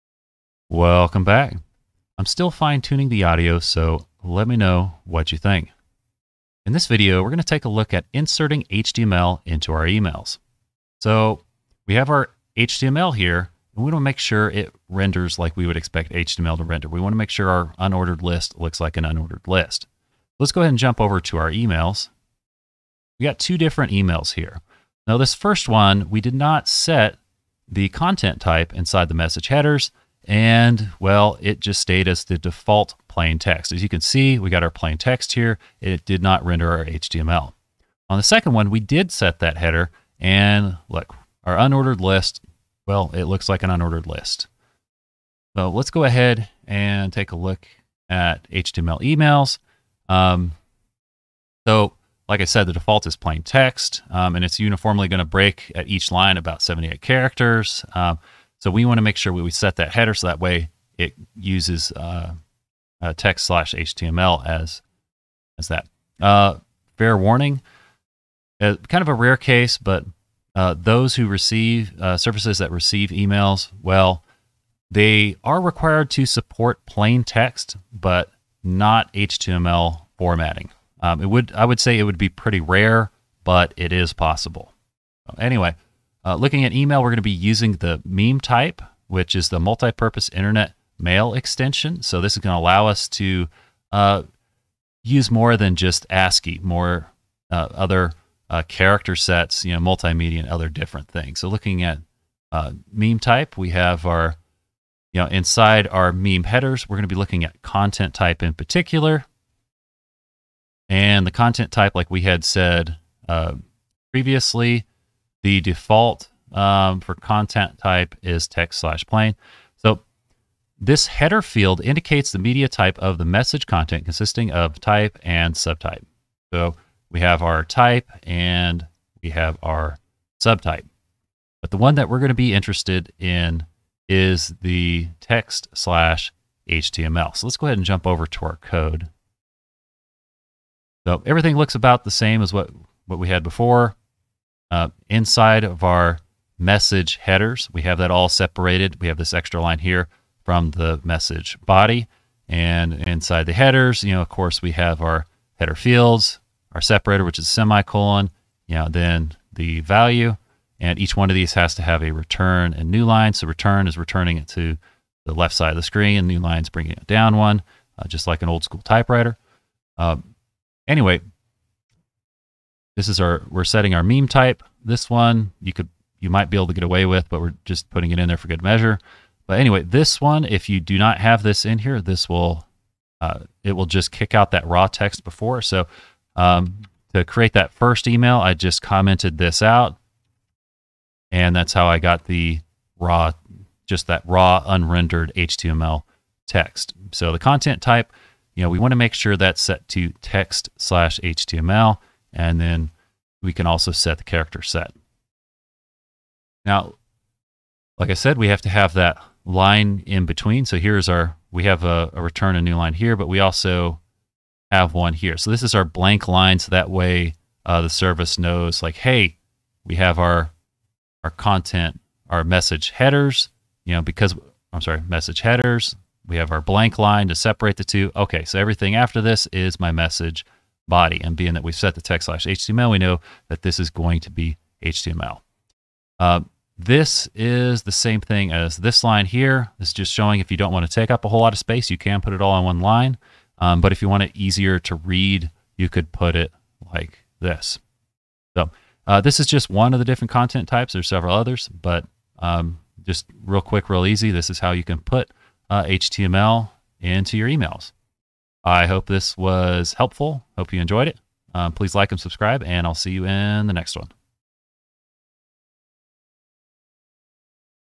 HTML in Your Emails - Golang Web Dev
In this video we cover how make sure HTML is rendered in emails.  The basic structure of an email is briefly explained as well.